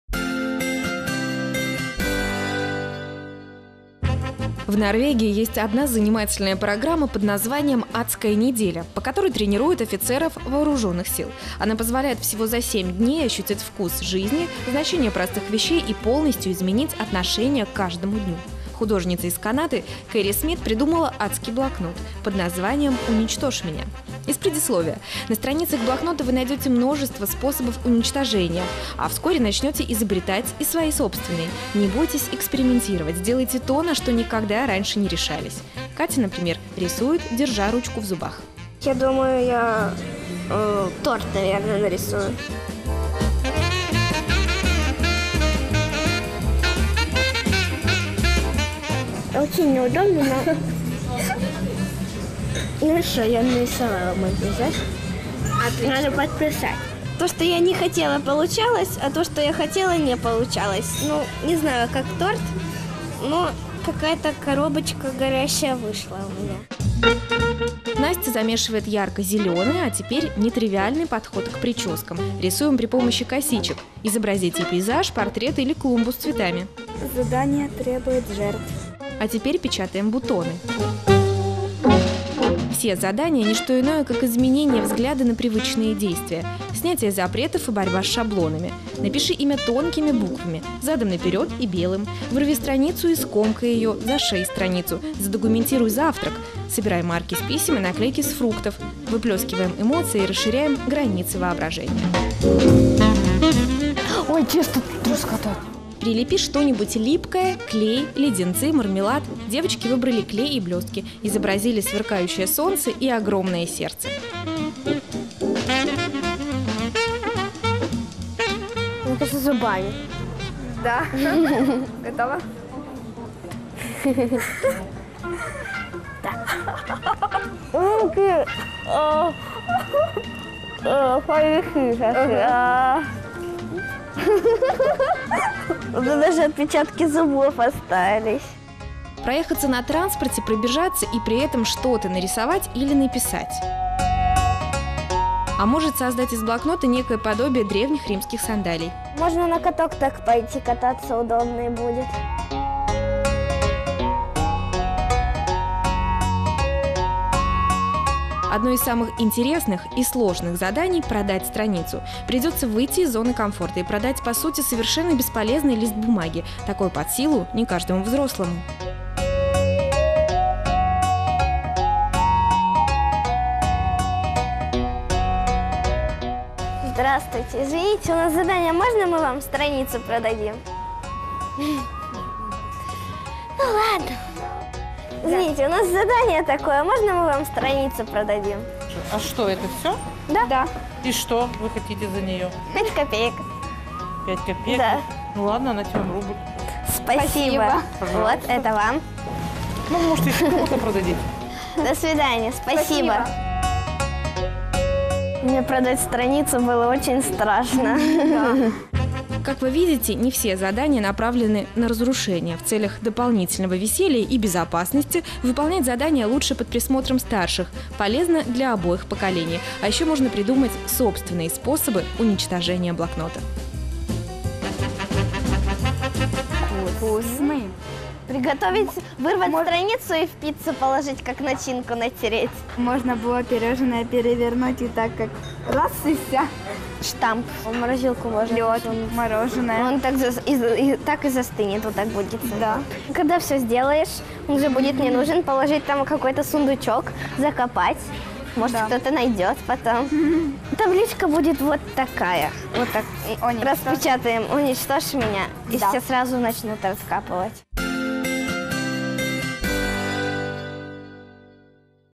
Уничтожь меня! Уникальный блокнот для творческих людей
"Уничтожь меня!" Эта книжка дает прекрасную возможность проявить свою творческую натуру, забыть предрассудки и страхи и окунуться с головой в безудержное творчество. Следуй предложенным советам или придумай собственные – и ты получишь колоссальное удовольствие от процесса! Рви бумагу, пачкай чернилами, вырезай фигуры – в этом блокноте можно делать все!